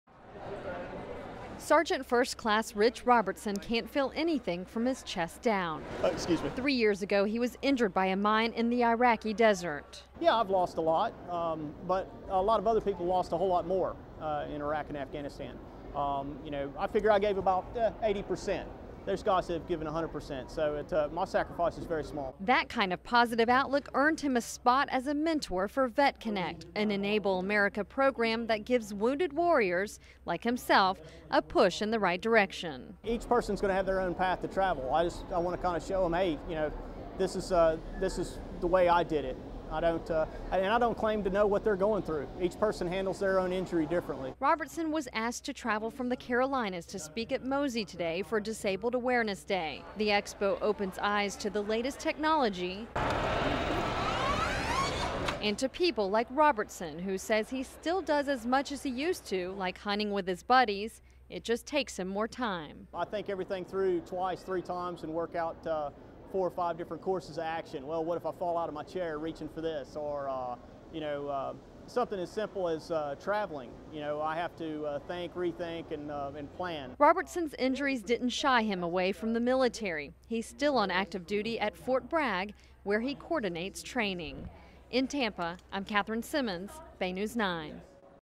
Enable America - VetConnect on Bay News 9
VetConnect mentor Rich Robertson interviewed by Bay News 9 while speaking at MOSI Disability Awareness Day